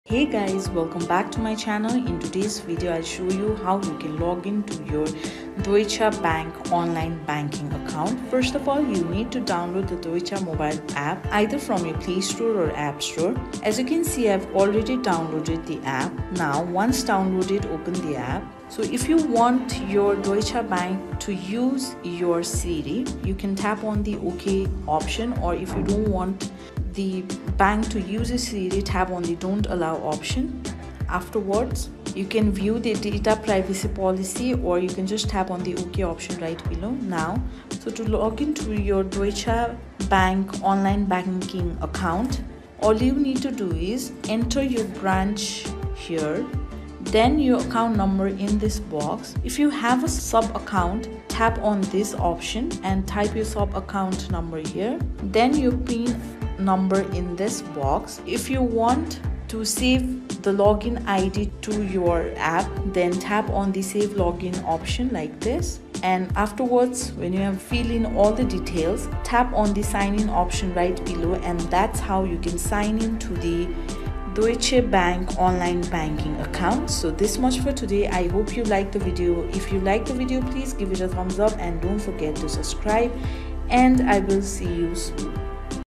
Deutsche Bank Online Banking | Login | Sign In
In this video i will guide you in quick easy steps to login to your Deutsche bank online banking account from the mobile app.